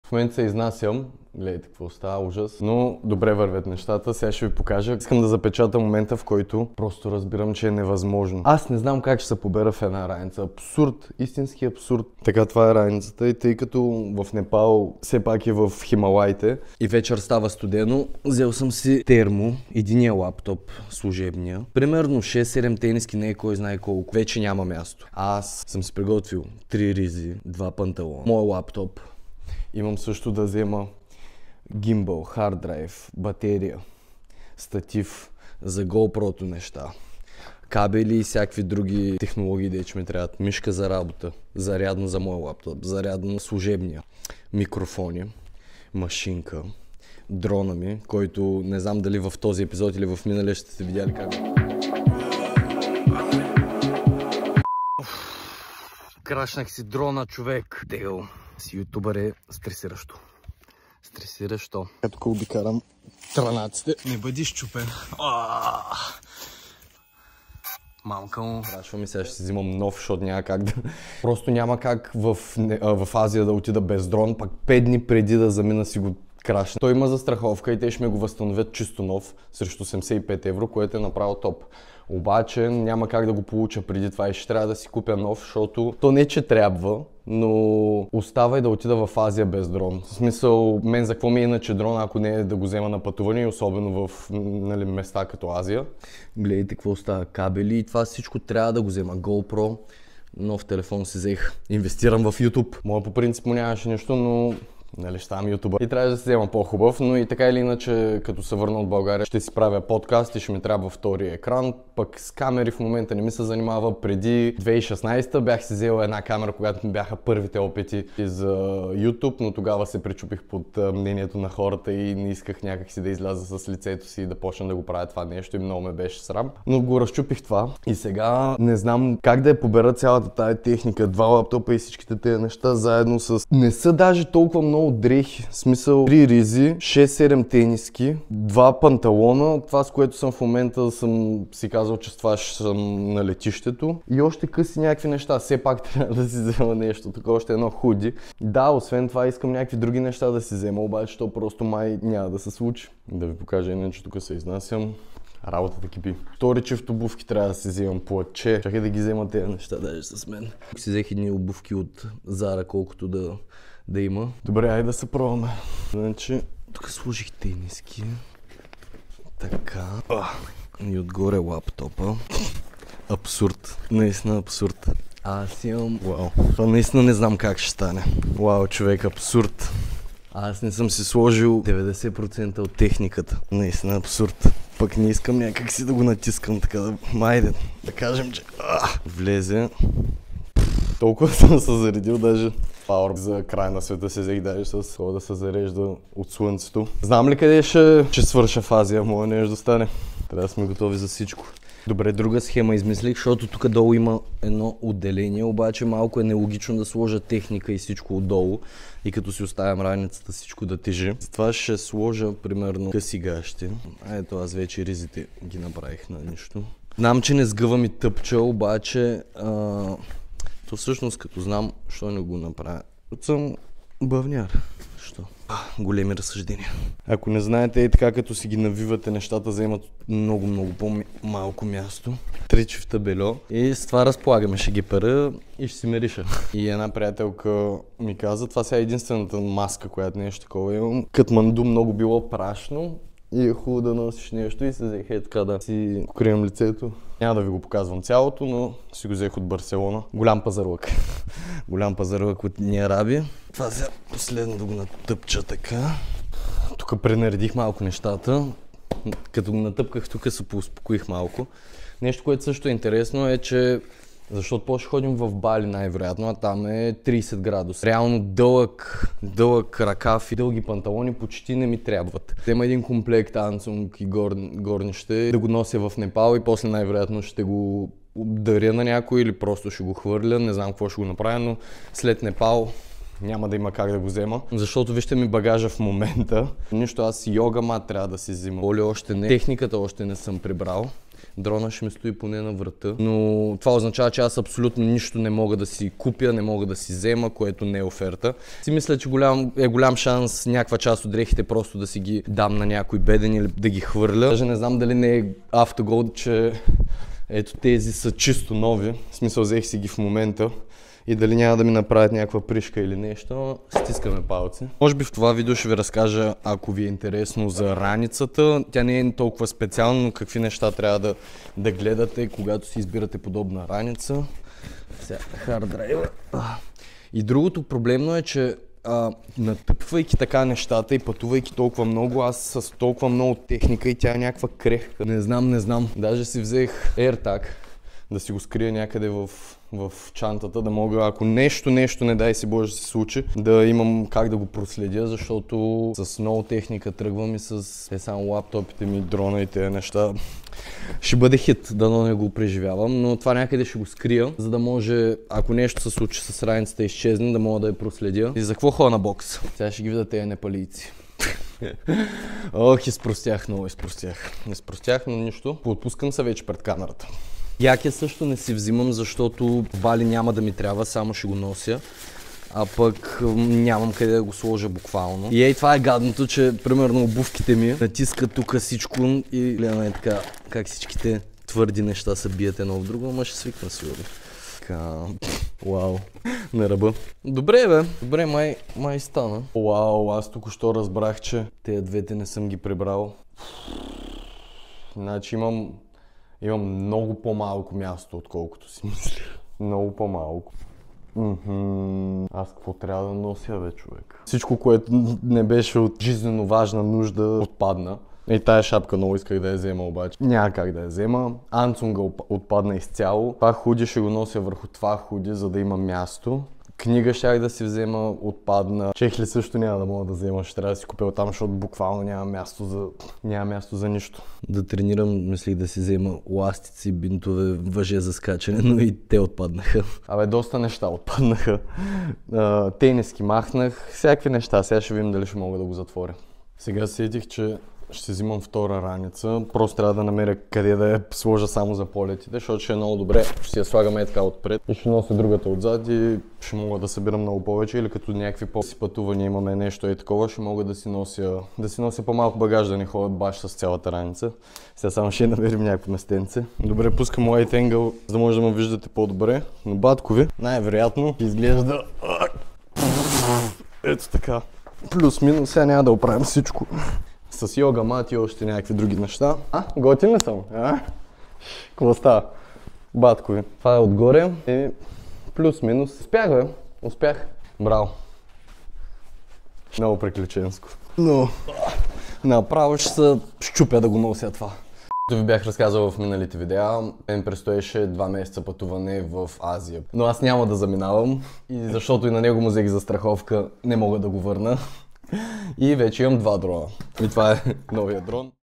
3 МЕСЕЦА САМО С РАНИЦА | Какво ще взема с мен?
Здравейте, приятели. Днес ви показвам какво ще нося в раницата си на пътуването ми в Бали и Непал,. Това ще бъде първото ми самостоятелно пътуване и нямам търпение.

Важно:
1. Най-голямото отделение да може да се отваря освен отгоре на раницата, но да може и отдолу или да има цип, за да не трябва да си изваждаш целия багаж ако имаш нещо на дъното.
2. Максимален брой джобове и места за предмети по самата раница.
3. Капацитет: 55 литра за мъж и 45 литра за жена имат оптимално съотношение тежест/капацитет. По-малки размери за дълги пътувания са просто недостатъчни.
4. Задължително да може да се пристяга към кръста и колкото повече могат да се регулират презрамките е по-добре, за да намериш оптималното разпределение на тежестта за твоето тяло.
5. Твърд гръб на раницата е предимство.
6. Втора по-малка раница, която да носиш на гърдите си може да бъде от полза, за да съхраняваш важни, чупливи или предмети, които трябва да вади постоянно от основната раница. Втора раница също така е полезна на по-кратки пътувания и ще ти позволи да не носиш целия си багаж навсякъде.